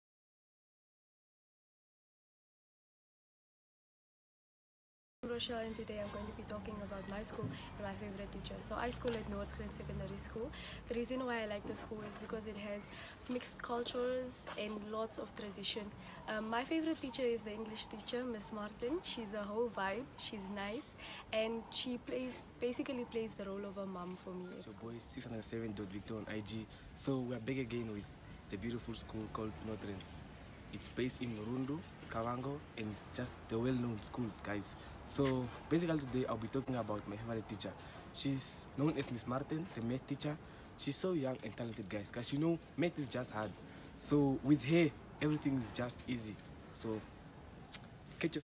My Zone TV: Noordgrens Secondary School
Learners from Noordgrens Secondary School in Rundu this week are sharing some exciting things they like about their favorite teacher at their school.